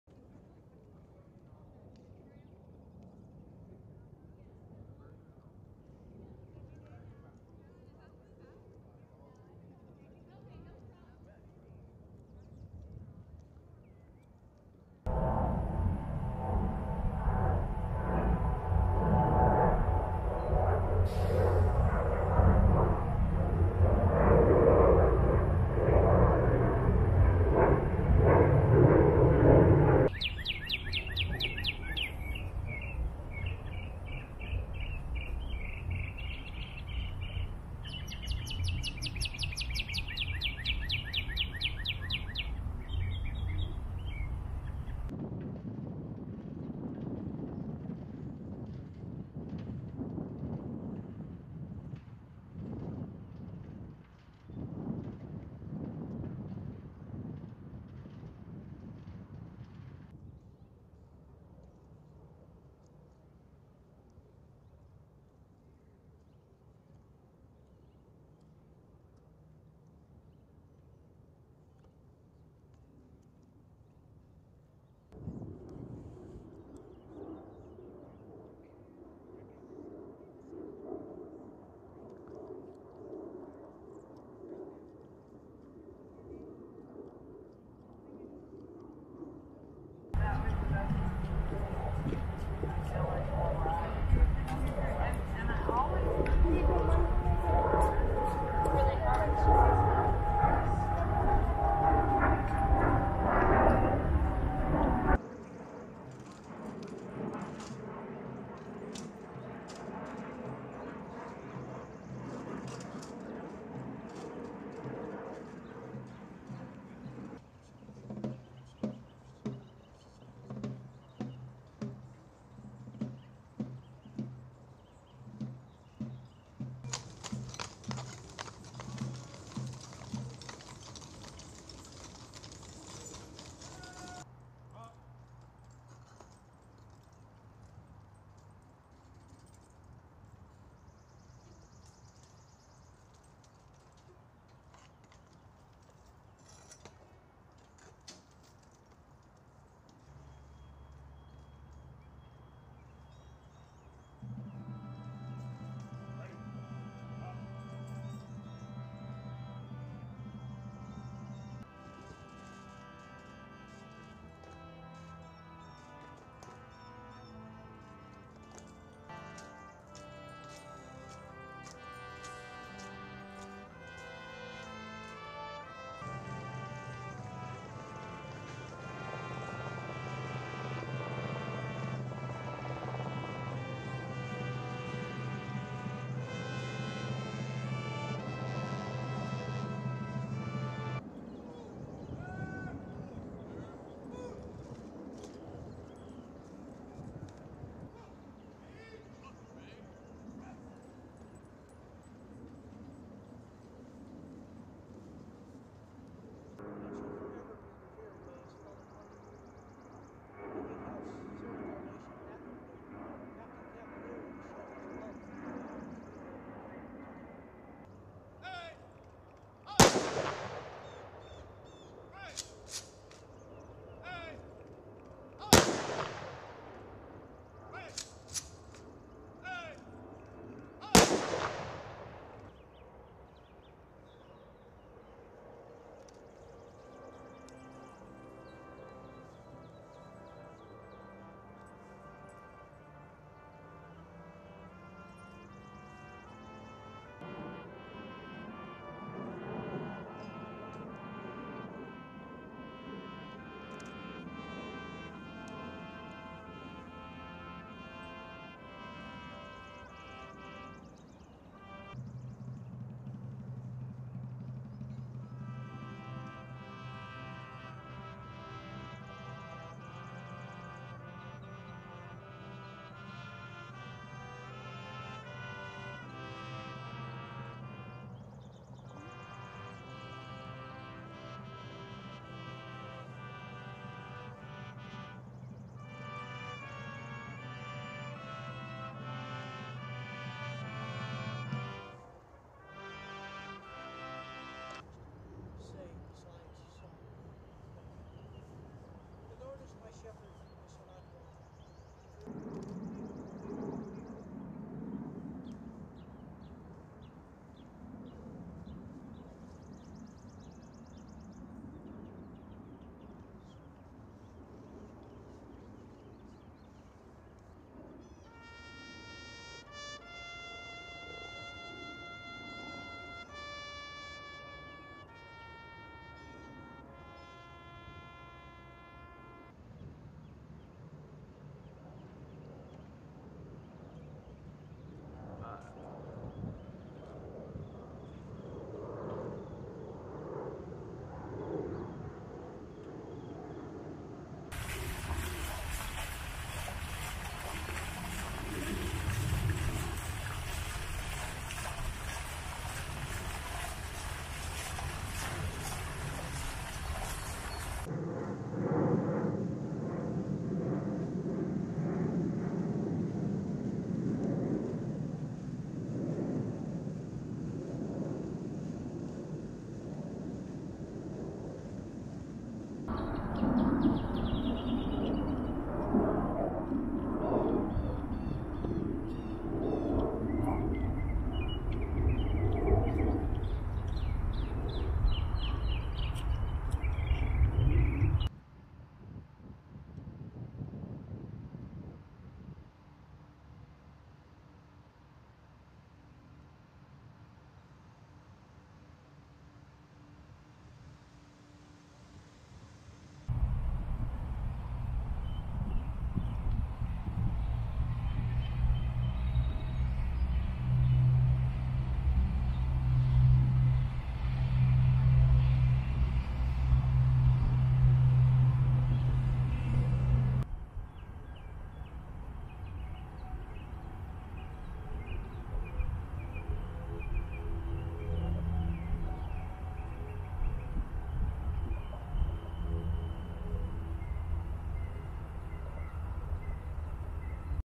Spring 2022 B-Roll - Arlington National Cemetery
This b-roll package includes video of the columbarium courts, the Eternal Flame of John F. Kennedy's gravesite, the Memorial Amphitheatre, the Mast of the USS Maine, a U.S. Army funeral service as well as images from the cemetery grounds.

ARLINGTON, VA, UNITED STATES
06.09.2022

An assortment of b-roll shots taken during the summer of 2021 at Arlington National Cemetery.

U.S. Army Video by Vincent Pecoraro / Arlington National Cemetery / Released

Film Credits: Video by Vincent Pecoraro
Arlington National Cemetery